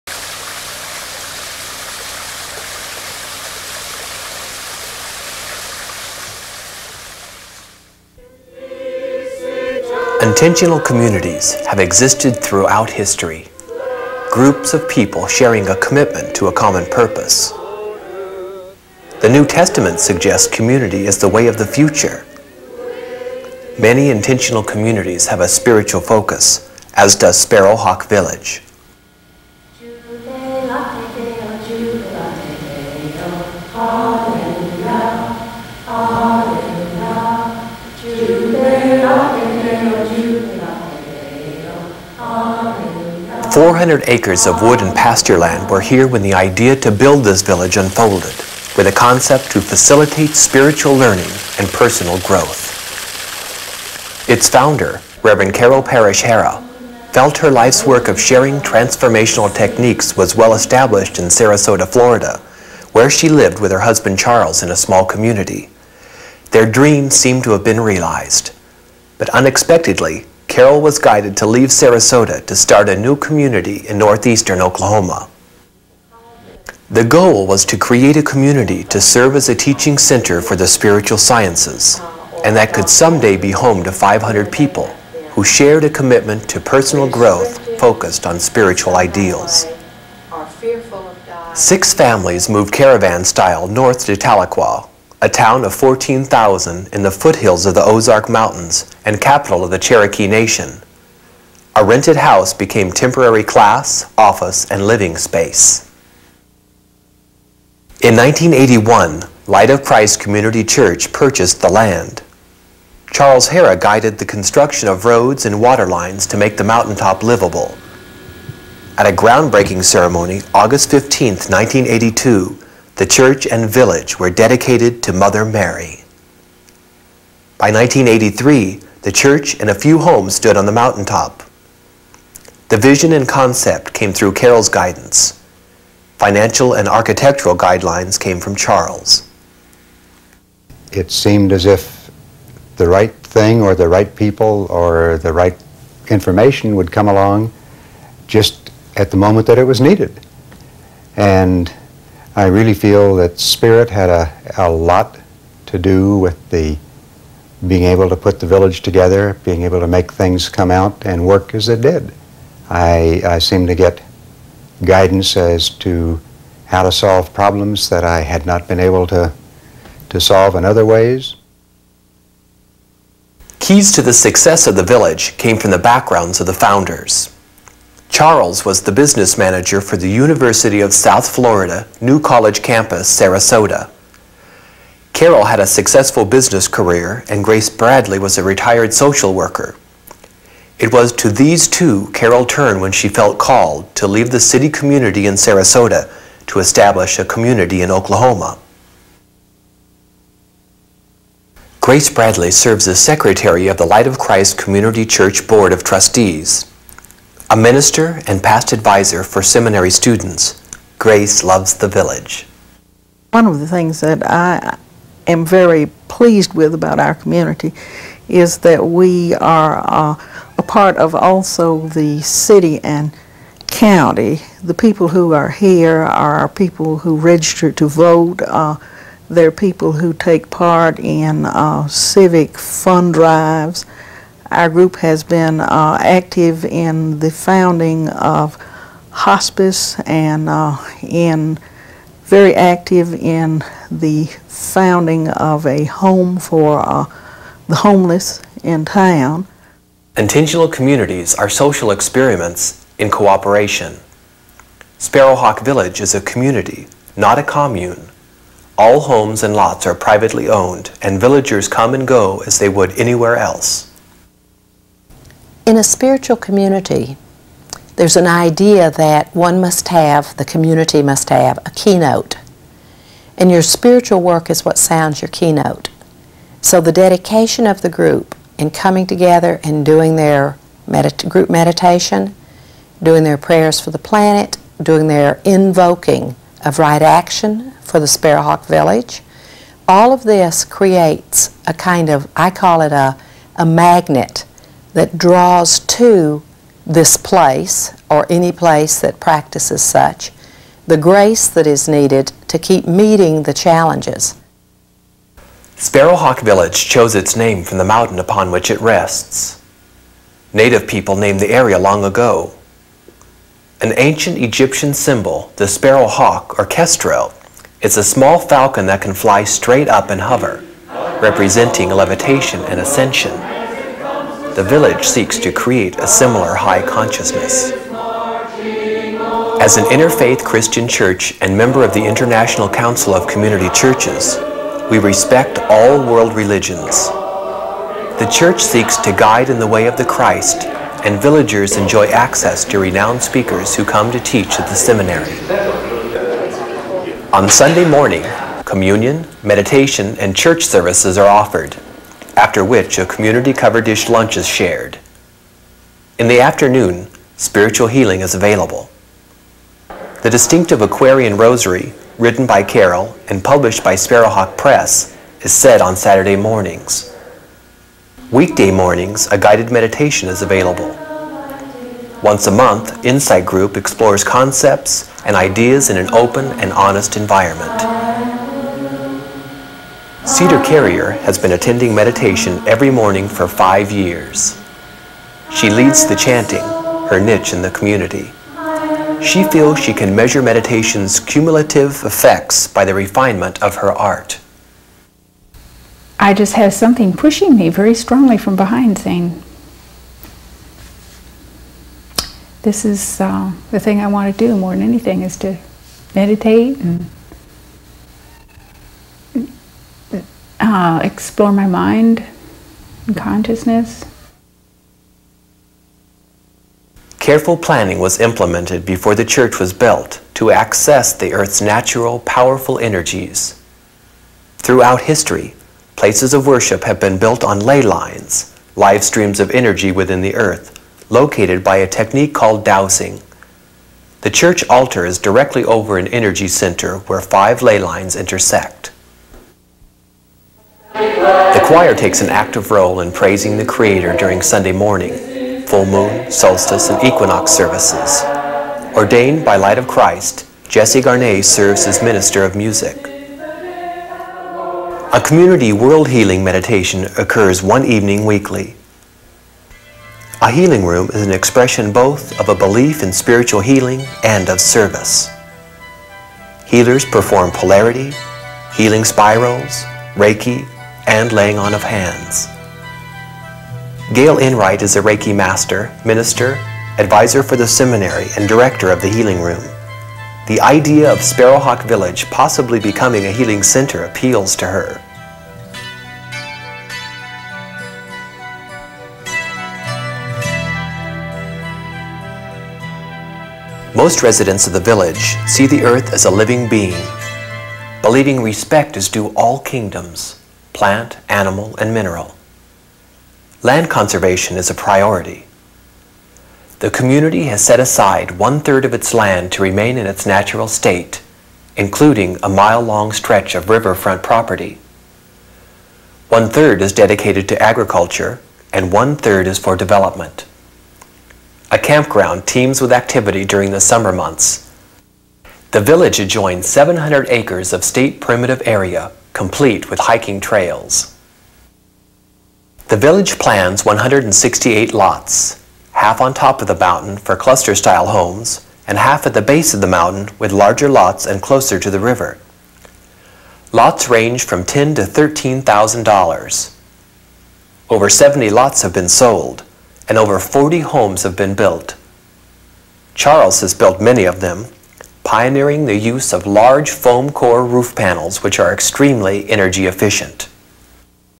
Sparrow Hawk Village 10 Year Anniversary (part 1)
This video made in 1991 celebrates the 10 year anniversary of Sparrow Hawk Village of Tahlequah Oklahoma; an intentional community and home of Sancta Sophia Seminary and Light of Christ Community Church. Part 1 of 2.